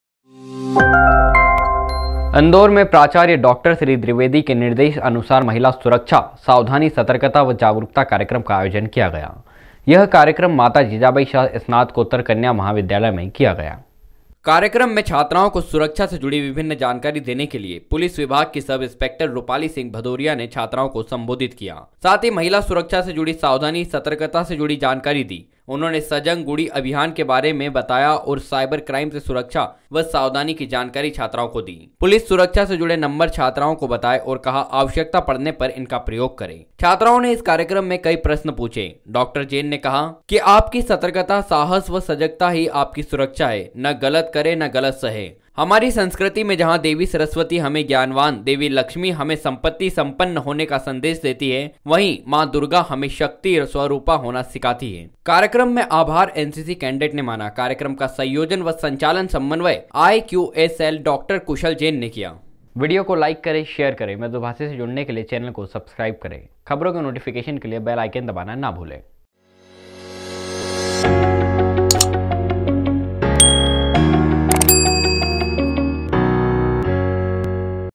सतर्कता व जागरूकता कार्यक्रम का आयोजन किया गया, छात्रों को मिली महिला सुरक्षा की सीख | Mradubhashi
इंदौर में प्राचार्य डॉ श्री व्दिवेदी के निर्देशानुसार महिला सुरक्षा सावधानी, सतर्कता व जागरूकता कार्यक्रम का आयोजन किया गया। यह कार्यक्रम माता जीजाबाई शा स्नातकोत्तर कन्या महाविद्यालय में किया गया।